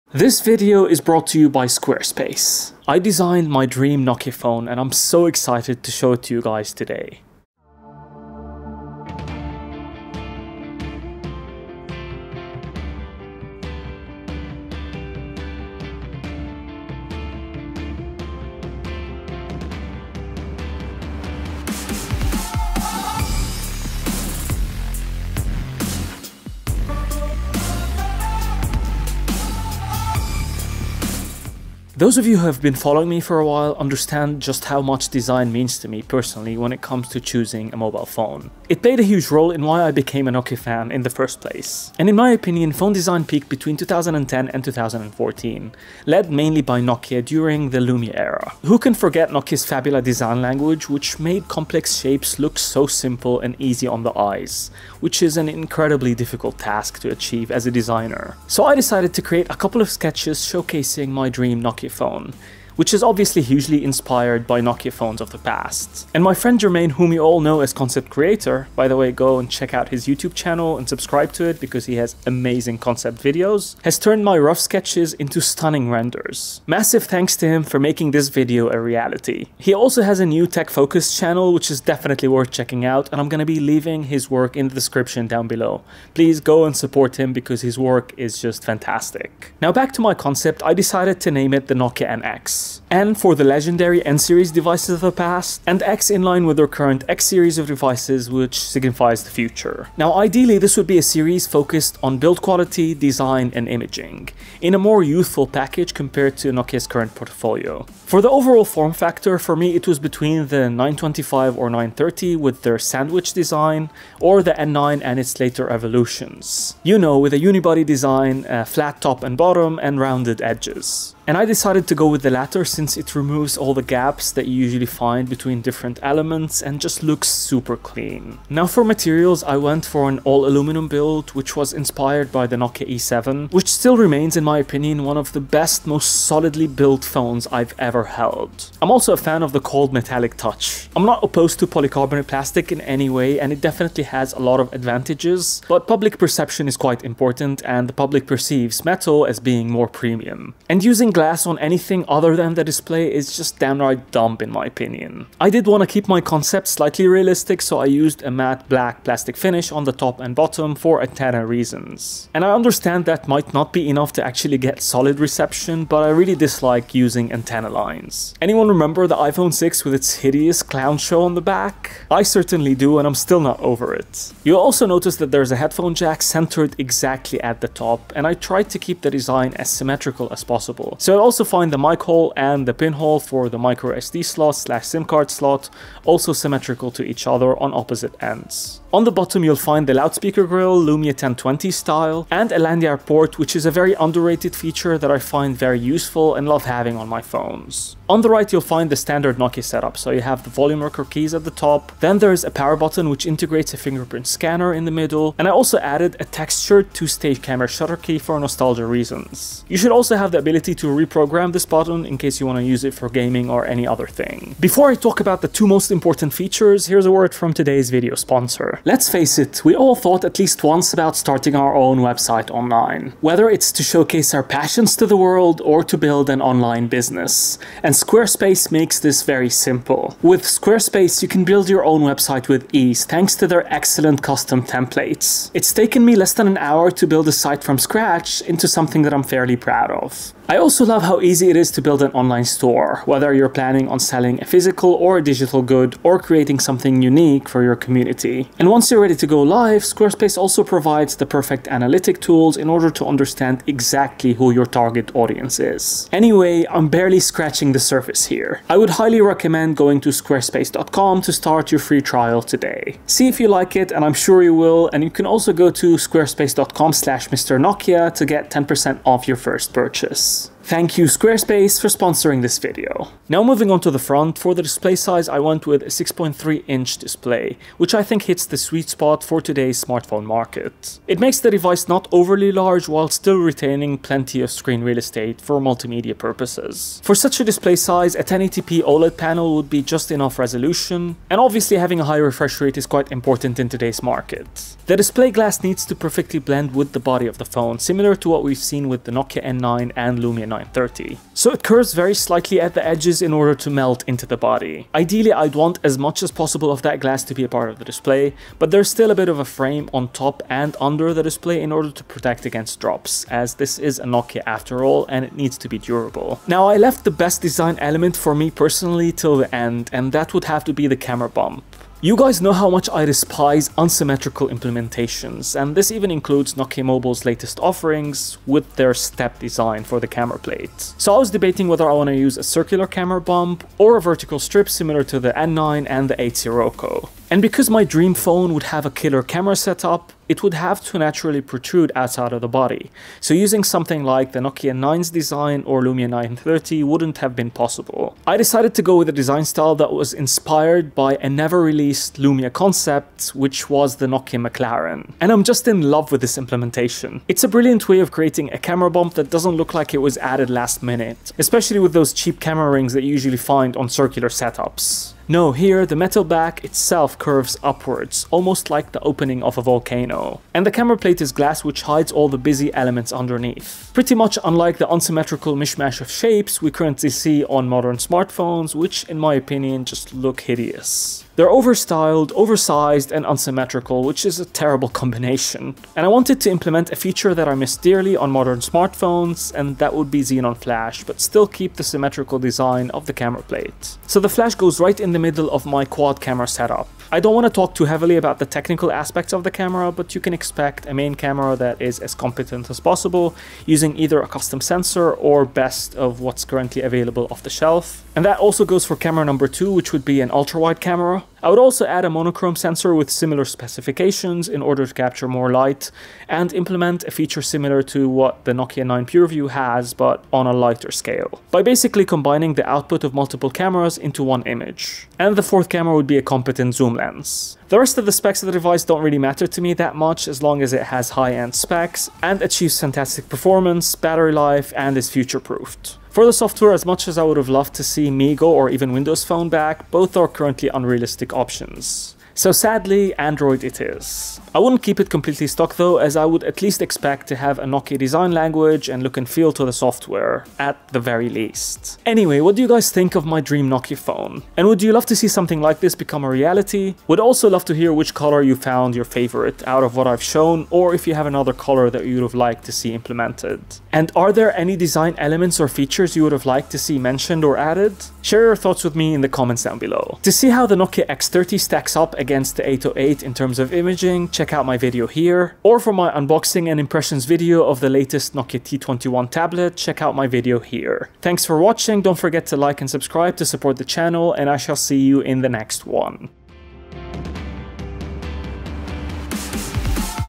Nokia NX | My Amazing Nokia Concept Phone
In this video I talk about my ultimate Nokia phone, the Nokia NX which was designed by me and made into a beautiful render by @Conceptcreator !

The Nokia NX is inspired by the best Nokia phones of the past in terms of design, following the fabula design principles first seen on the Nokia N9. It features a 6.3" OLED display, a quad camera setup on the back and runs on Android with a Nokia flavour to it :)

00:00 - Renders
00:31 - Introduction
01:29 - Why Nokia NX?
01:47 - Form Factor
02:08 - Build & Materials
02:35 - Hardware Tour
03:34 - Squarespace
04:33 - Display
05:16 - The Cameras!
07:07 - Specs & Software
07:31 - Do you like it?!